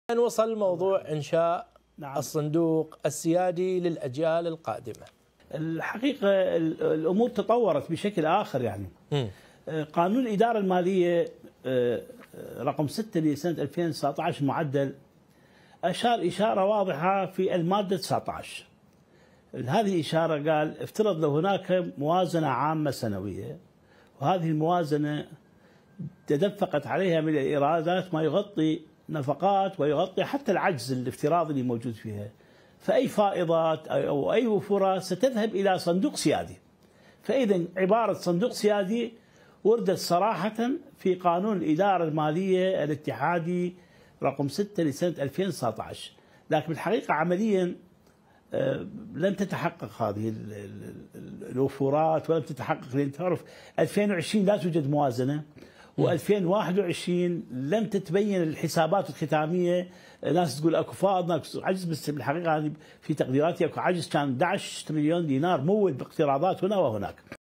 أحمد الصفار: أدرجنا في موازنة 2019 ضرورة إنشاء صندوق سيادي
عضو مجلس نواب السابق أحمد الصفار: أدرجنا في موازنة 2019 ضرورة إنشاء صندوق سيادي